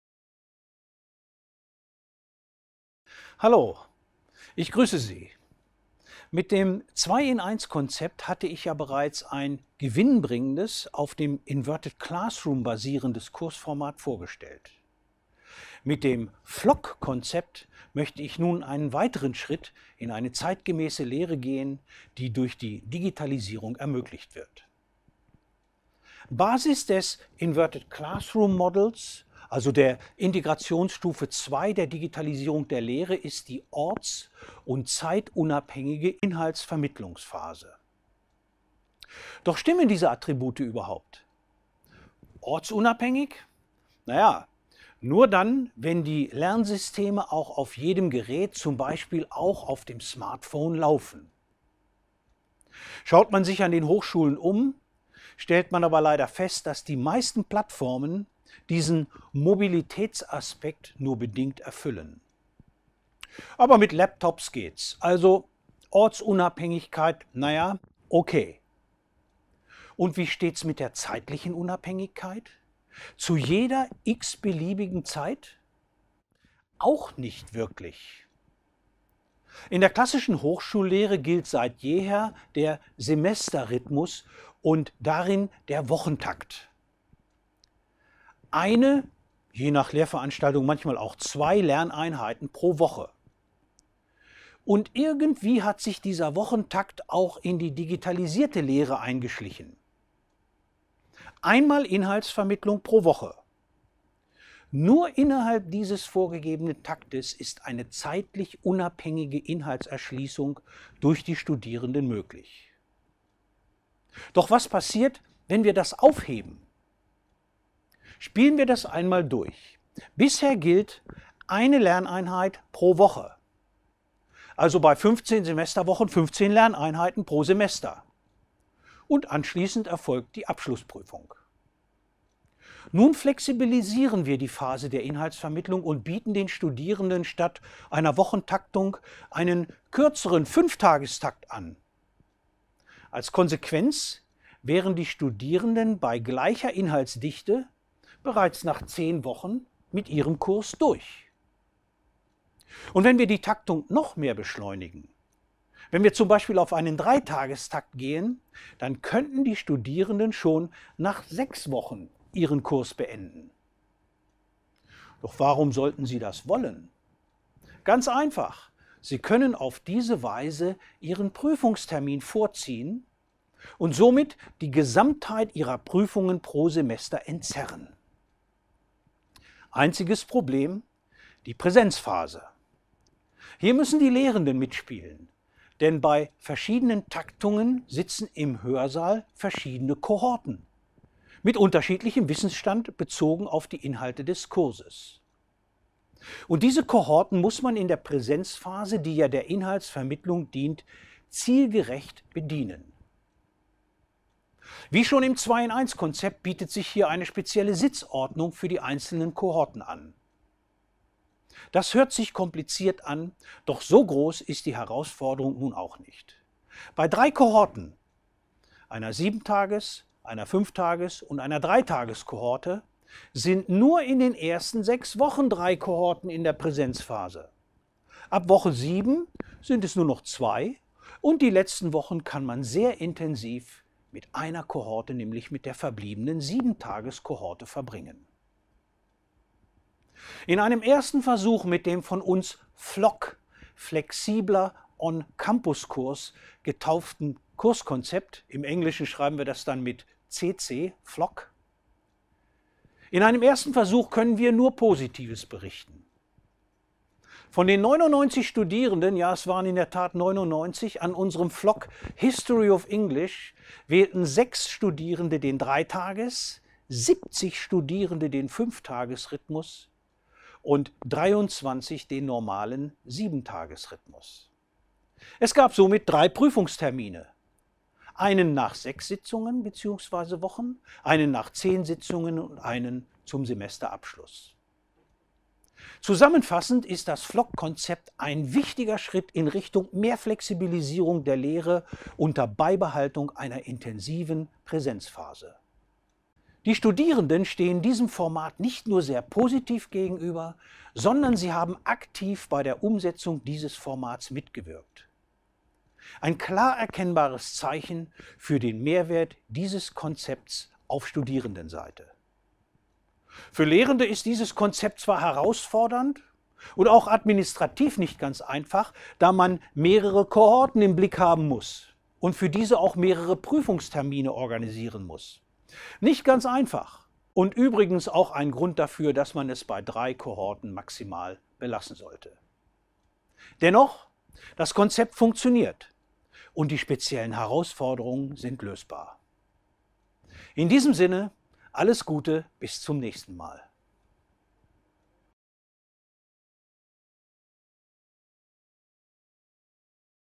Das FLOCK Konzept
Der Flexible On Campus Kurs (FLOCK) ist die jüngste Innovation des Teams um Prof. Handke, das die Vortteile des digitalen Lehrens und Lernens auf Lehrveranstaltungen mit Präsenz anwendet. Das Modell, das nur als Inverted Classroom Variante funktioniert, erlaubt den Studierenden eigene Taktungen bei der digitalen Inhaltsvermittlung und ermöglich so flexiblere Termine für die Abschlussprüfung.